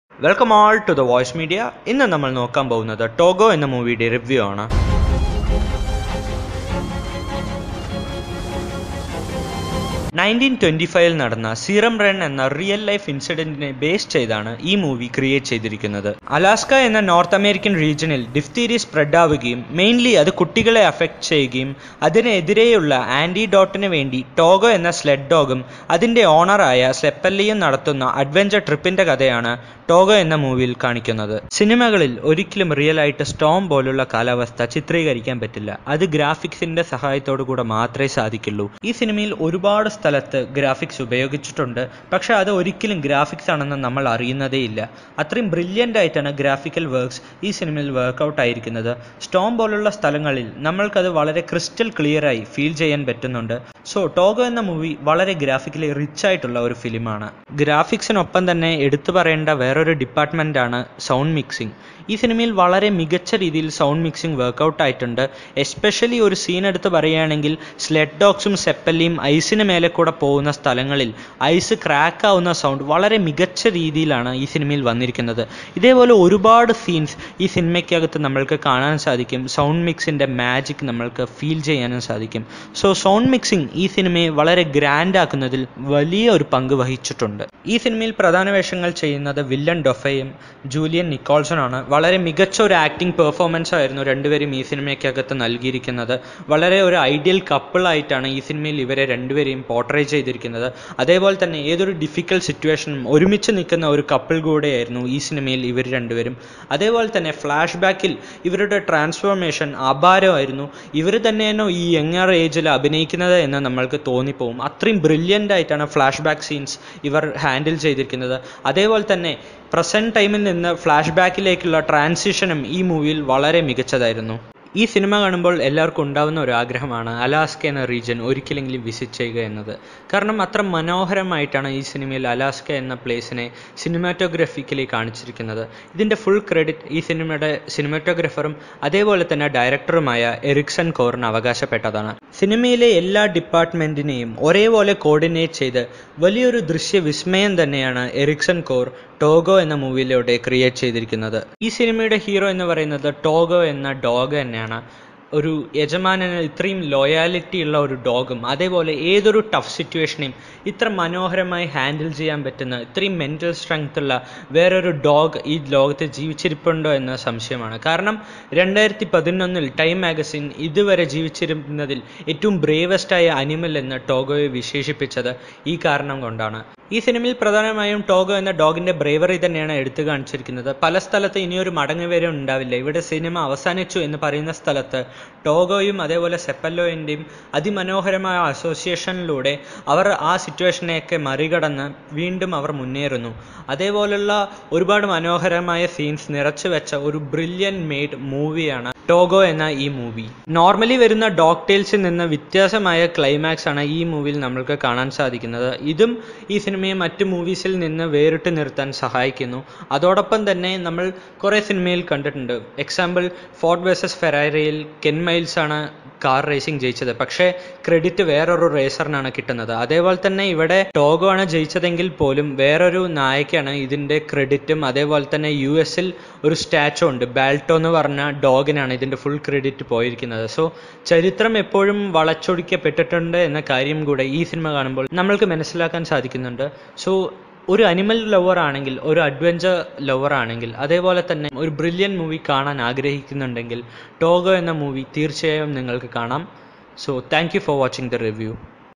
Togo Movie Malayalam Review
The tale of the sled dog, Togo, who led the serum run in 1925, but was deemed too small and frail by most to lead such an intense race.

The Voice Media